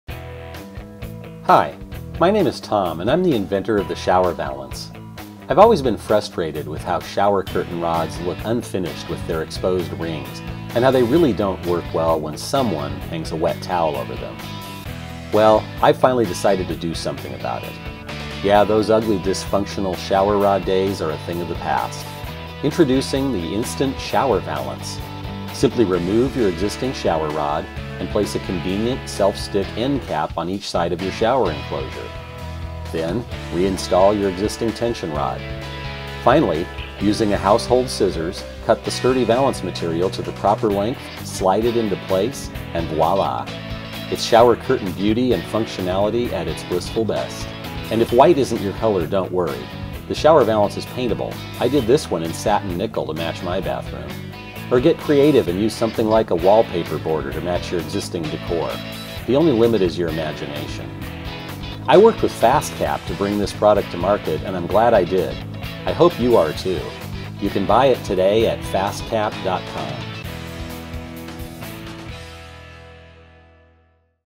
Introducing the Shower Valance by FastCap
Instantly turn your existing shower rod and curtain into a beautiful
and functional enhancement to your bathroom decor.